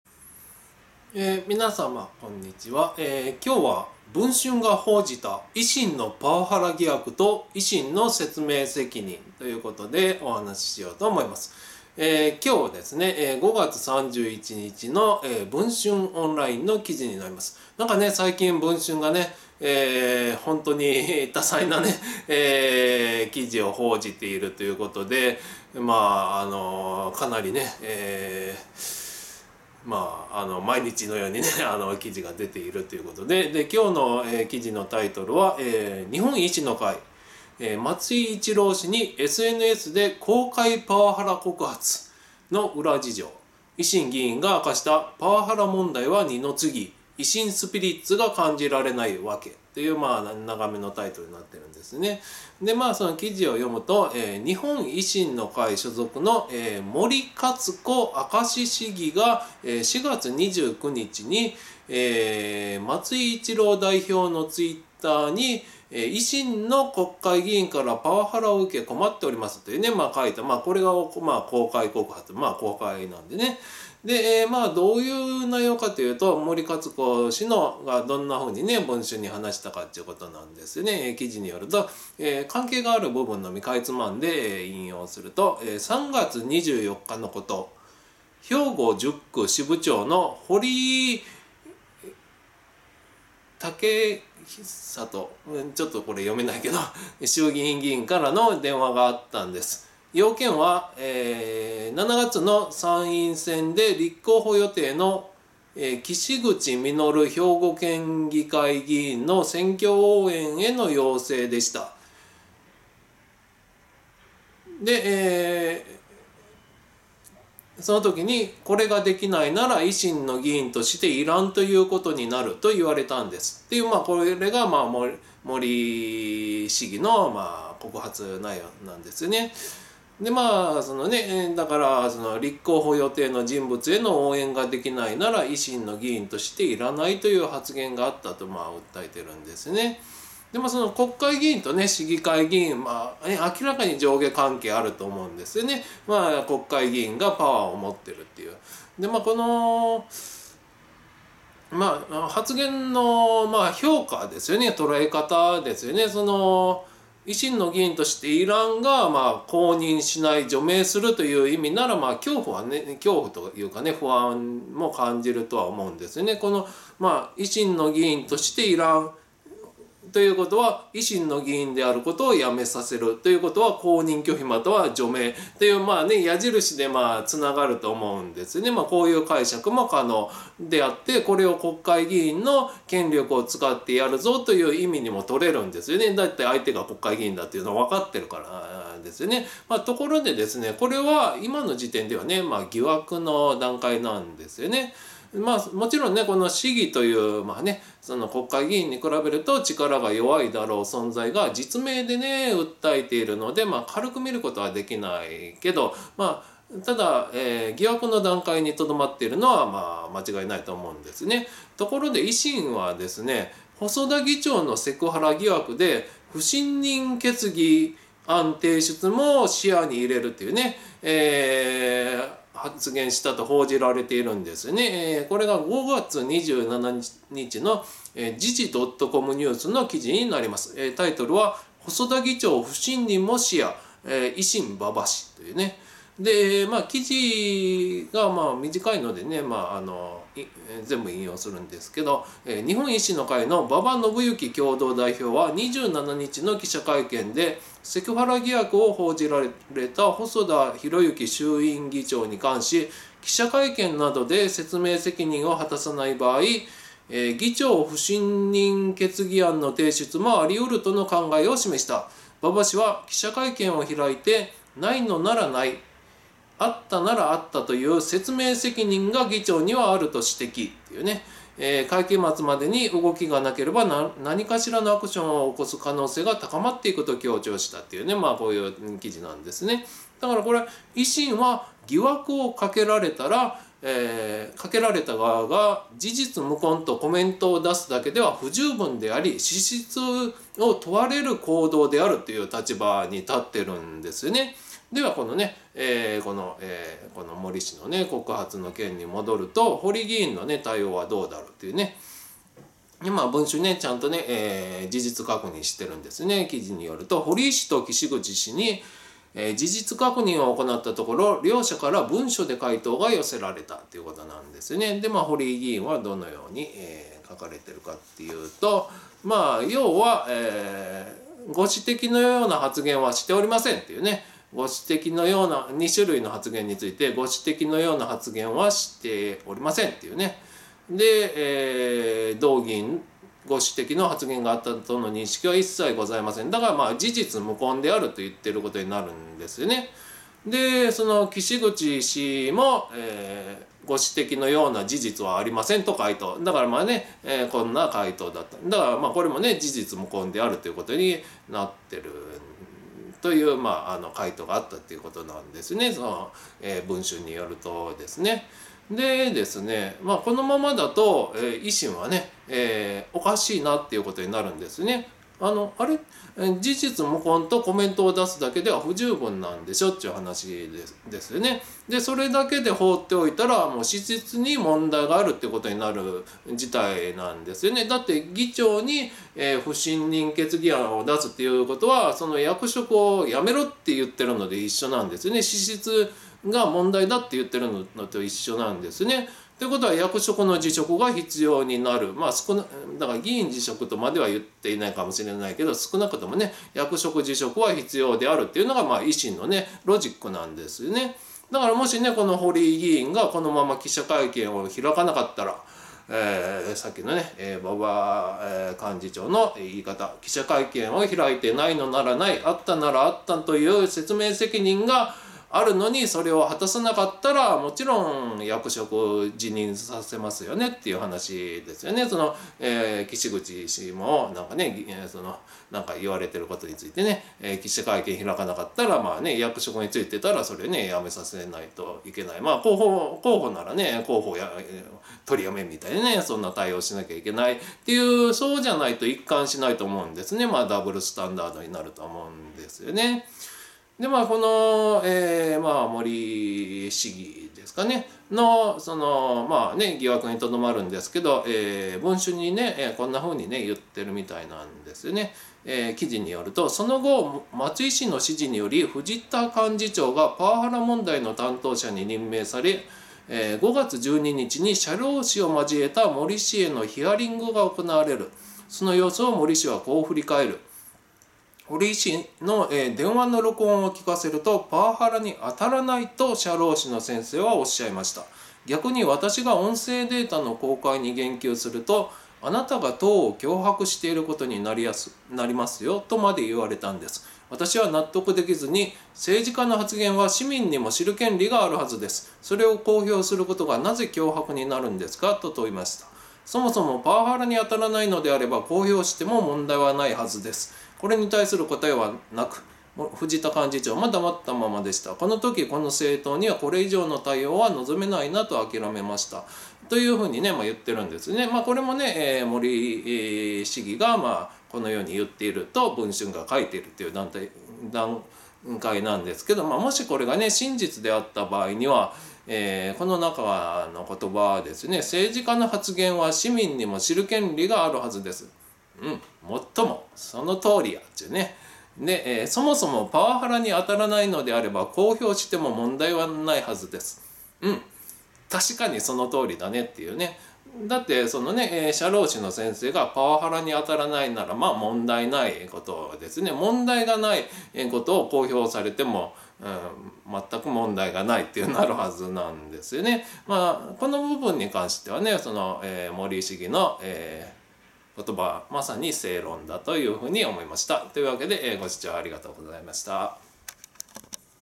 文春が報じた維新のパワハラ疑惑と維新の説明責任
今日、５月３１日の文春オンラインの記事。
『日本維新の会・松井一郎氏にSNSで“公開パワハラ告発” の裏事情　維新議員が明かした「パワハラ問題は二の次」「“維新スピリッツ”が感じられない」ワケ』
『「…3月24日のこと…、兵庫10区支部長の堀井健智衆議院議員から電話があったんです。…要件は7月の参院選で立候補予定の岸口みのる兵庫県議会議員の選挙応援への要請でした。…『これができないなら維新の議員としていらんということになる』と言われたんです」』
立候補予定の人物への応援ができないなら、維新の議員としていらないという発言があったと森議員は訴えている。
国会議員と市議会議員。上下関係。国会議員がパワーを持っている。
「維新の議員としていらん」が、公認しない・除名するという意味なら、恐怖を感じるだろう。
「維新の議員としていらん」→「維新の議員であることをやめさせる」→「公認拒否又は除名」
これを、国会議員の権力を使ってやるぞという意味にもとれる。
ところで、これが、現時点では疑惑。
市議という国会議員と比べて力が弱いだろう存在が、実名で訴えているので、軽く見ることはできないが、疑惑の段階。
ところで、維新は、細田議長のセクハラ疑惑で、不信任決議案提出も視野に入れると発言したと報じられている。
５月２７日の時事ドットコムニュースの記事。
『細田議長不信任も視野　維新・馬場氏』
『日本維新の会の馬場伸幸共同代表は２７日の記者会見で、セクハラ疑惑を報じられた細田博之衆院議長に関し、記者会見などで説明責任を果たさない場合、議長不信任決議案の提出もあり得るとの考えを示した。
馬場氏は「記者会見を開いて、ないのならない、あったならあったと言う説明責任が議長にはある」と指摘。「会期末までに動きがなければ、何かしらのアクションを起こす可能性が高まっていく」と強調した。』
維新は、疑惑をかけられたら、「事実無根」とコメントを出すだけでは不十分であり、資質を問われる行動であるという立場。
では、堀井議員の対応はどうだろう。
『堀井氏と岸口氏に事実確認を行ったところ、両者から文書で回答が寄せられた。』
堀井議員『森氏に対し、岸口氏への選挙応援を依頼し、それを拒めば「維新の議員としてはいられない」と発言したことについて以下のように回答した。 《…（発言については）ご指摘のような発言はしておりません》
4月3日に堀井氏が「（自分に歯向かってきたやつは）僕の目の前におれへんようになった。要するに全部除名してきた」と発言したという森氏の主張に対しては「ご指摘のような発言はしておりません」。
森議員がパワハラ被害を訴えていることに対し見解を聞くと、こう回答した。 《同議員ご指摘の発言があったとの認識は、一切ございません。…』
「事実無根である」と言っていることになる。
『岸口氏にも、2019年の統一地方選挙で森氏に怒鳴ったこと、2021年の県知事選挙でタコなどが入った段ボール箱を市民から受け取ったこと、および森議員がパワハラ被害を訴えていることの見解を聞いたところ、《ご指摘のような事実はありません》と回答した。』
このままだと、維新はおかしいことになる。
あれっ。事実無根とコメントを出すだけでは、不十分なんでしょ。
資質が問われる事態なんでしょ。
役職の辞職が必要なんでしょ。（議員辞職ではなくても）
堀井議員がこのまま記者会見を開かなかったら、役職辞任させますよね。
そうじゃないと、一貫しない。
ダブルスタンダードということになる。
『その後、松井氏の指示により藤田幹事長がパワハラ問題の担当者に任命され、5月12日に社労士を交えた森氏へのヒアリングが行われる。その様子を森氏はこう振り返る。
「堀井氏の電話の録音を聞かせると、パワハラにあたらないと社労士の先生はおっしゃいました。逆に、私が音声データの公開に言及すると、『あなたが党を脅迫していることになりますよ』とまで言われたんです。私は納得できずに『政治家の発言は市民にも知る権利があるはずです。それを公表することがなぜ脅迫になるんですか』と問いました。そもそもパワハラに当たらないのであれば、公表しても問題はないはずです。これに対する答えはなく、藤田幹事長も黙ったままでした。この時、この政党にはこれ以上の対応は望めないな、と諦めました」』